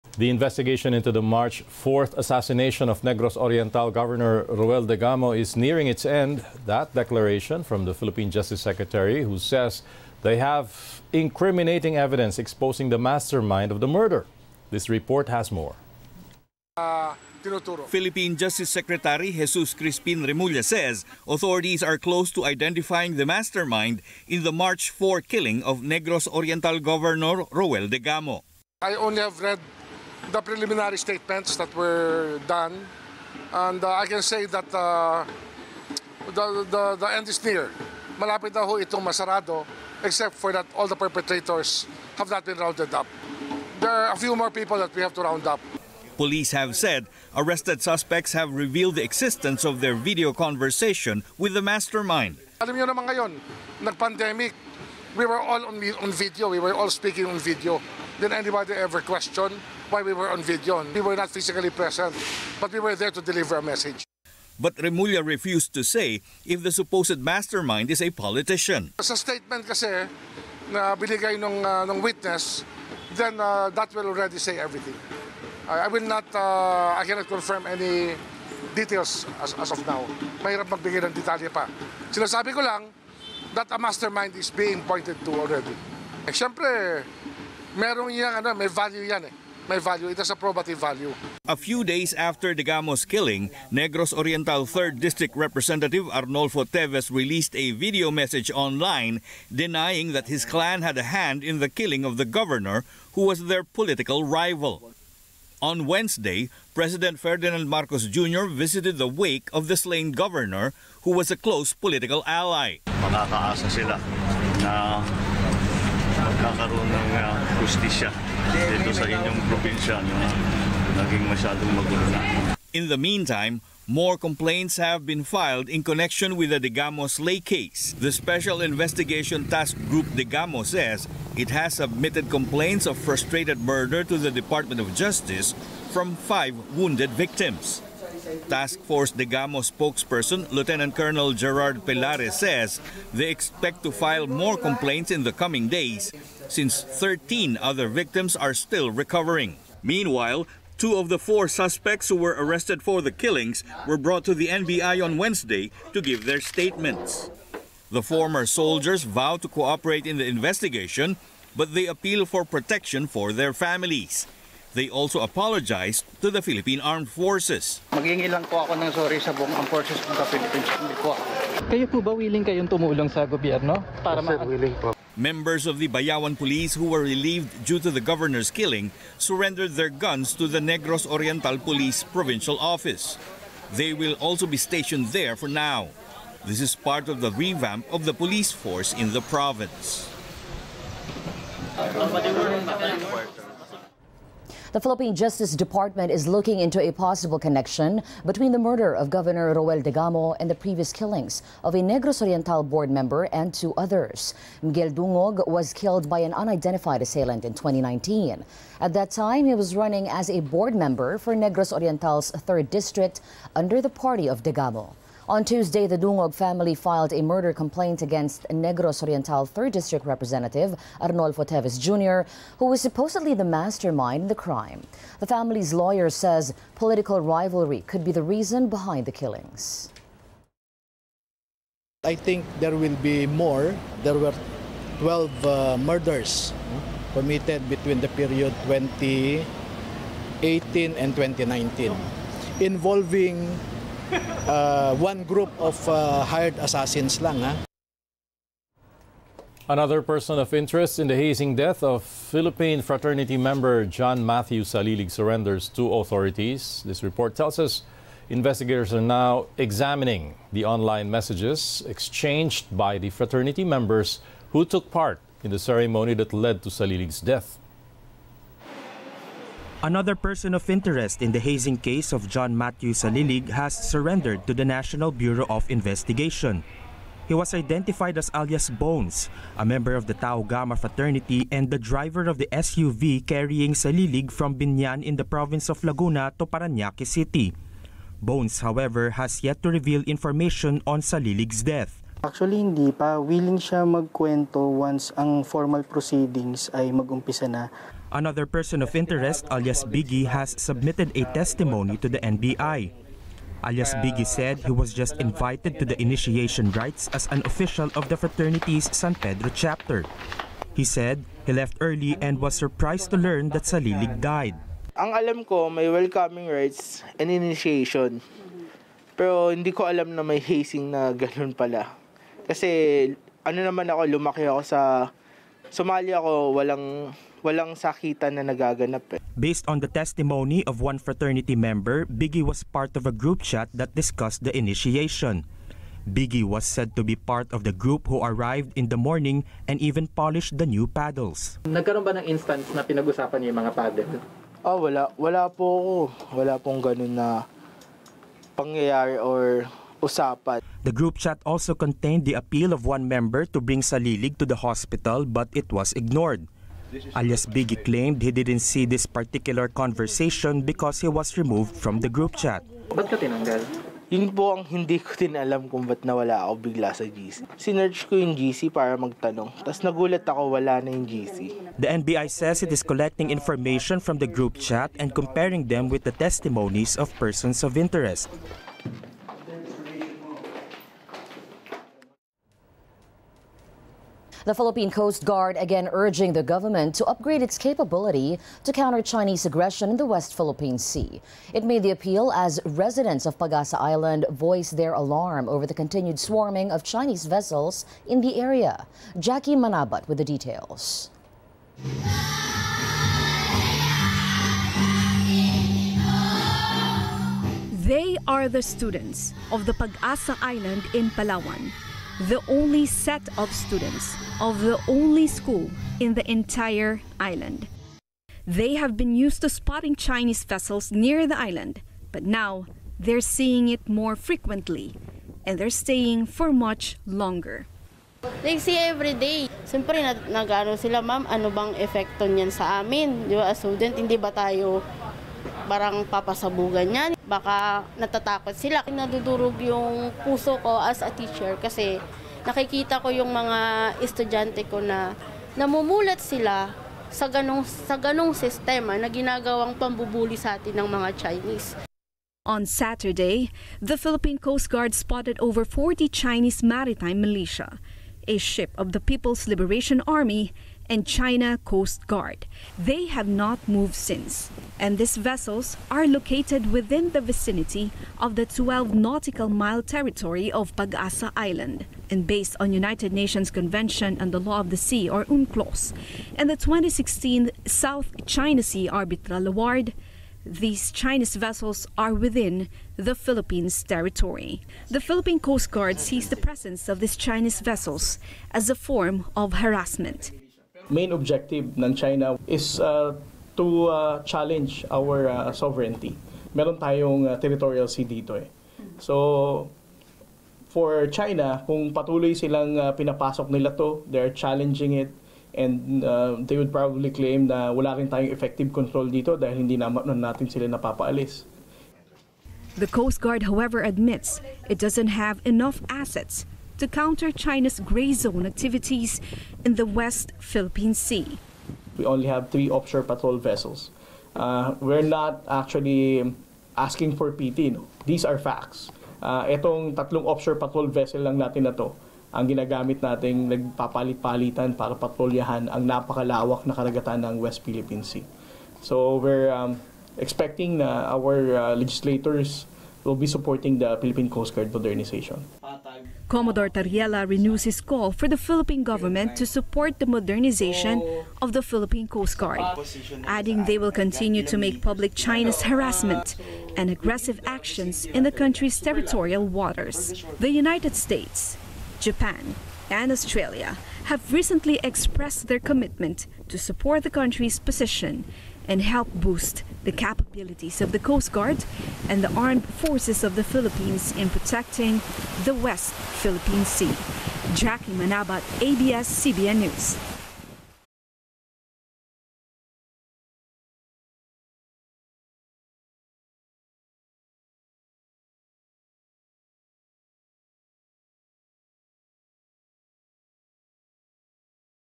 Marcos visits Degamo's wake | ANC
Rundown: The investigation into the March 4th assassination of Negros Oriental Governor Roel Degamo is nearing its end. That declaration from the Philippine Justice Secretary, who says they have incriminating evidence exposing the mastermind of the murder.